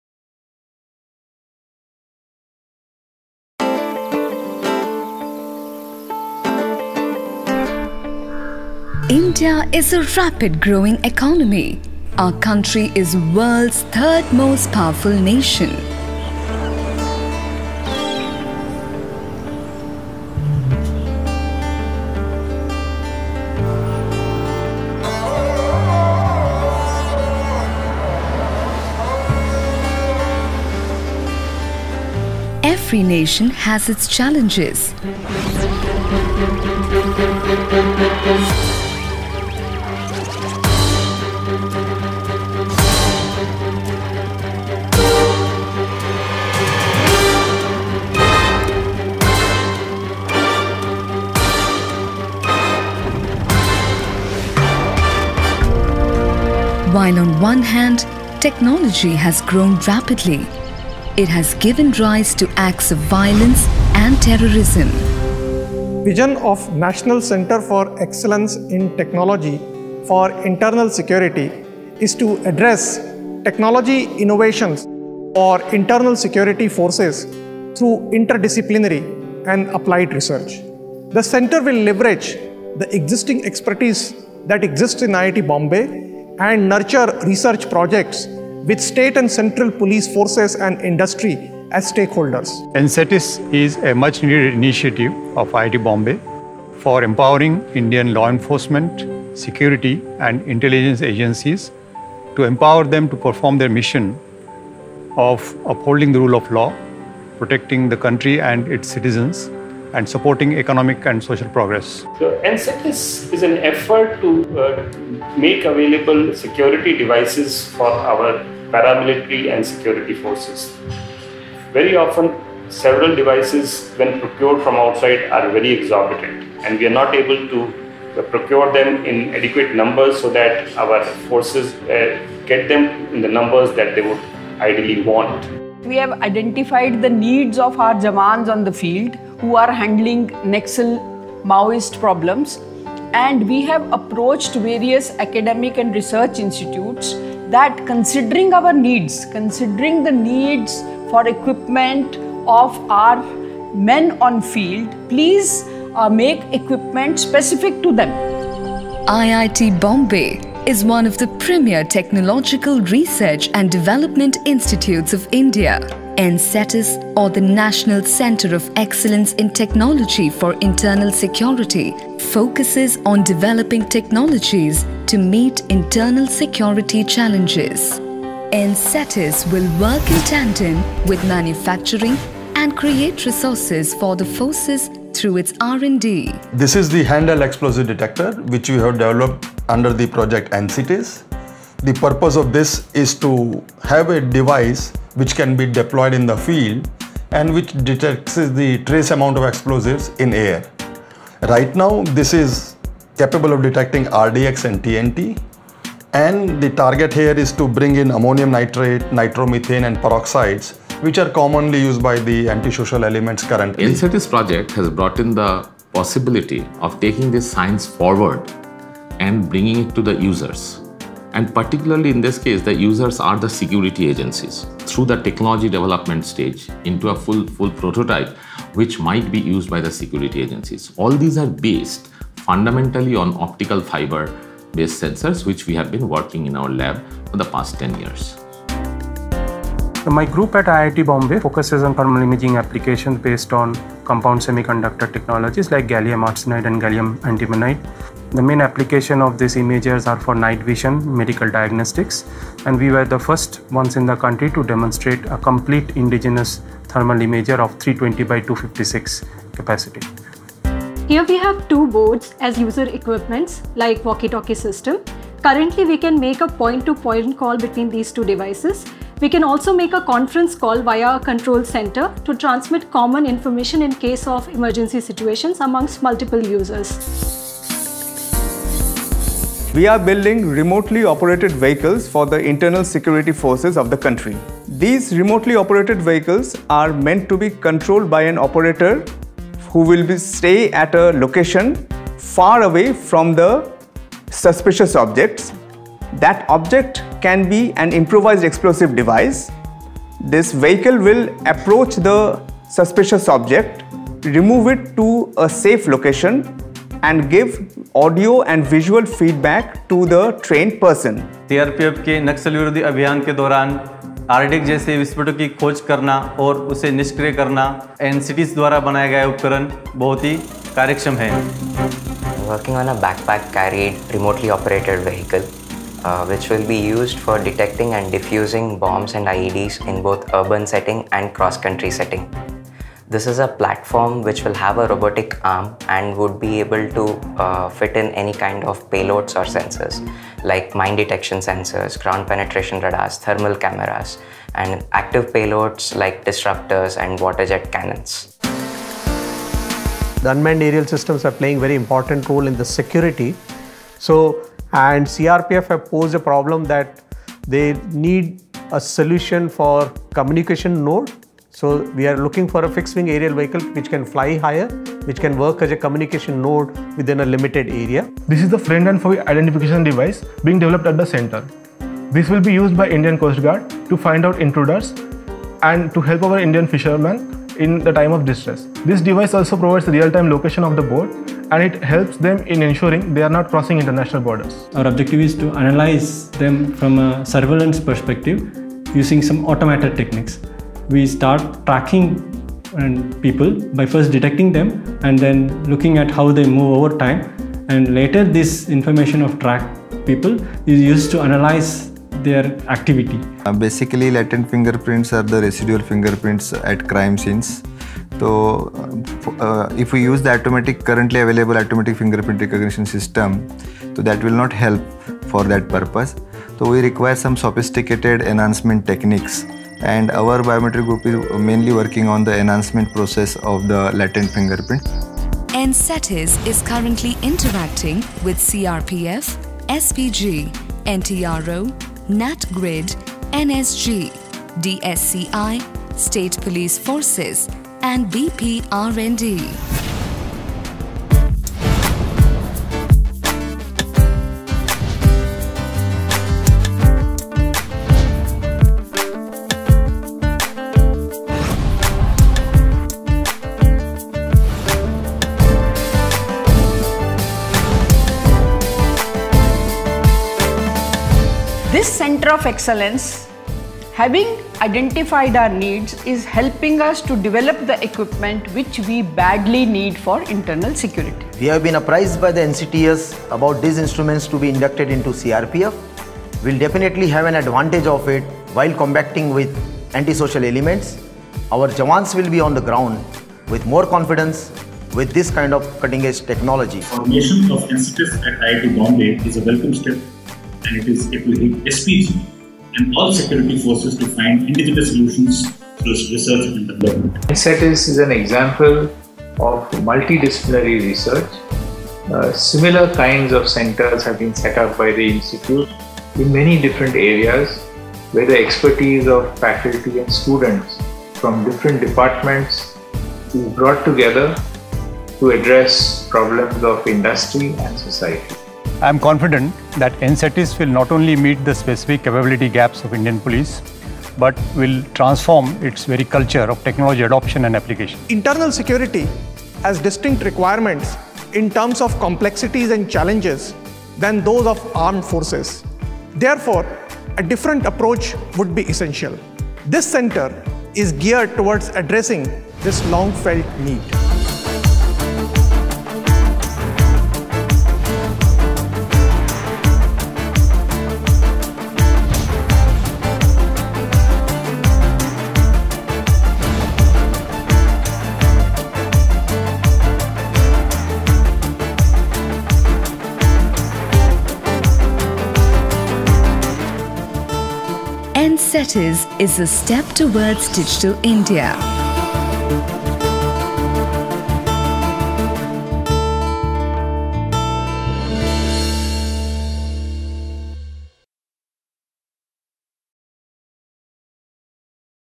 NCETIS -Digital India for paramiltary forces
National Center of Excellence in Technology for Internal Security (NCETIS) at Indian Institute of Technology Bombay has been setup under  Government of India's flagship Digital India program. It is targeted towards developing indigenous technology and self sufficiency in the broad areas of Electronics System Design Manufacturing (ESDM) in the strategic sector of internal security.

It has been felt that India needs a long term and focused approach to develop technology to meet its internal security challenges- specially to address the requirements of state and central police forces and agencies involved in public safety and disaster management. Internal security has distinct requirements in terms of complexities and challenges as the threats arise on account of various factors such as social structures and inherent inequities; therefore, a different approach would be essential.  NCETIS is expected to address this national requirement.

Shot and Directed by Krishand
Editor  Rakesh Cherumadam
Music Ajmal Hasbullah
Associate director Vishnu Haridas
Motion graphics  Daksha Animation studio
Motion templates - Envato
Motion graphic artist  - Manu Sankar
Animation creative director - Nitin Anand
Sound Design and Mixing - Sam Santosh
Animation executive producer - Deljo Thekkekara
Voice over artist - Neelima Devdas
Camera and Equipment Rental - Accord Equipment, Book my lens
Project Coordination (NCETIS SIDE) - Seema Periwal, Smita Narwade
Production Management - Rakhil Akkali
Special Thanks - Syama Bindu, Rahul Bhasarkar, Jishnu Krishnan, prof. Sudesh Balan sir , prof Abhay Karandirkar